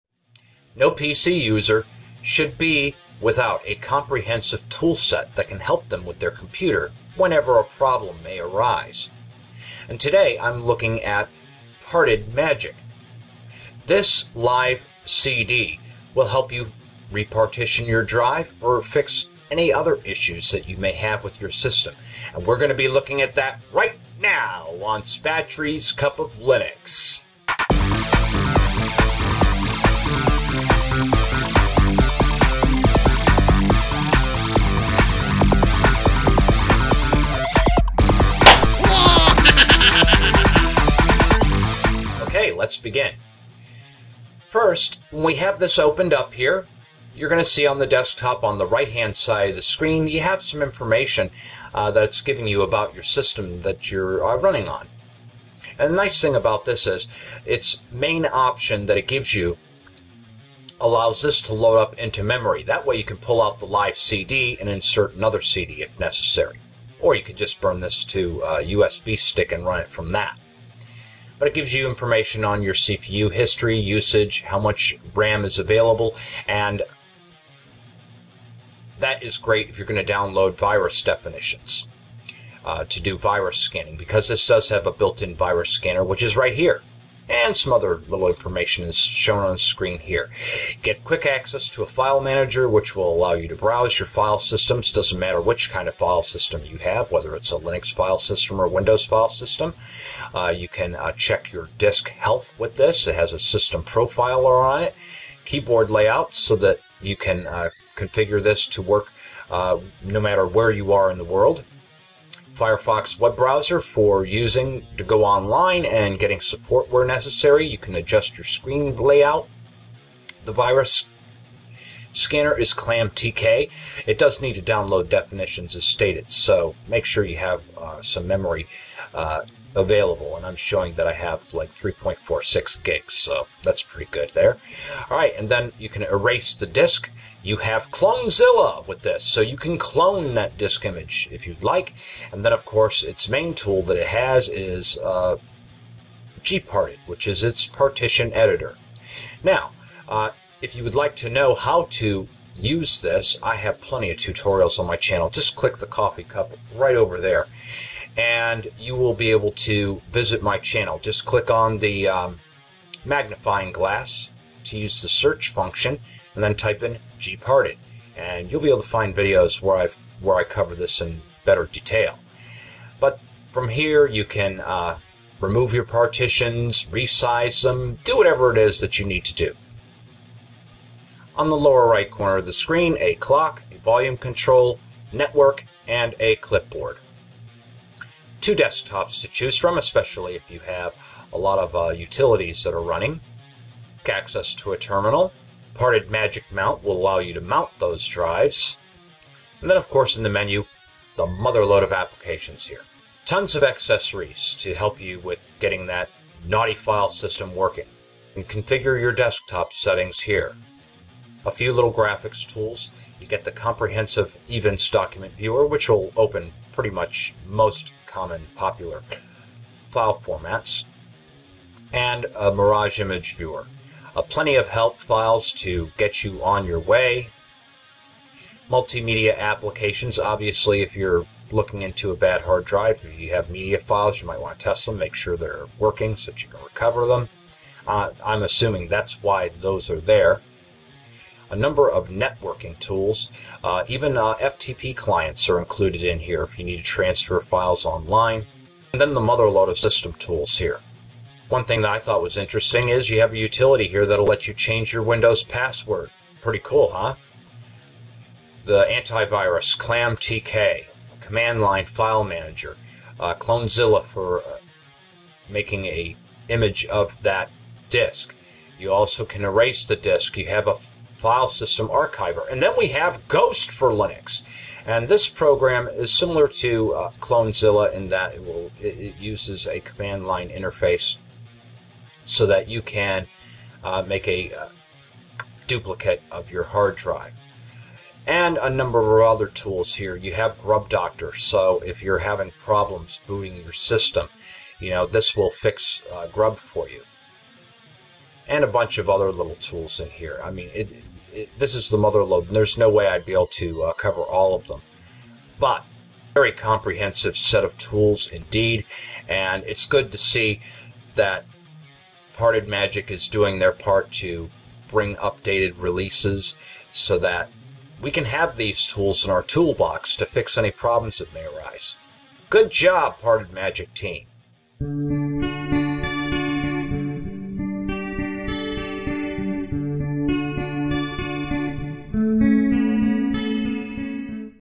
Review ::: Parted Magic 2013-6-14
Parted Magic is a small live CD/USB/PXE with its elemental purpose being to partition hard drives. Although GParted and Parted are the main programs, the CD/USB also offers other applications, such as Partition Image, TestDisk, fdisk, sfdisk, dd, ddrescue, etc.